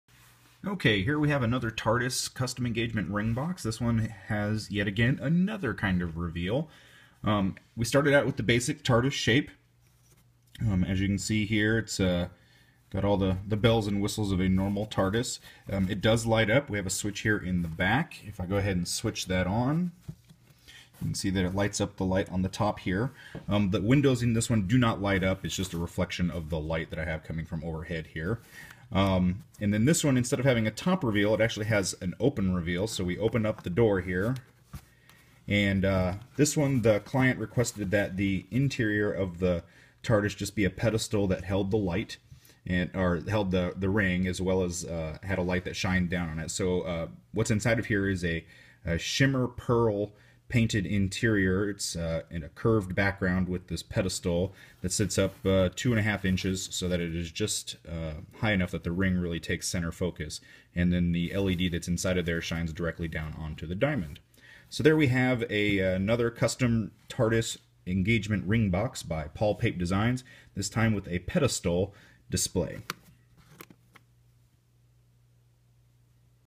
Doctor Who TARDIS Custom Engagement Ring Box
This TARDIS from Dr. who is a custom engagement ring box by Paul Pape Designs. The reveal on this one is that the doors open and inside is a shimmery column in which the rings sits on top of it with a light shining down.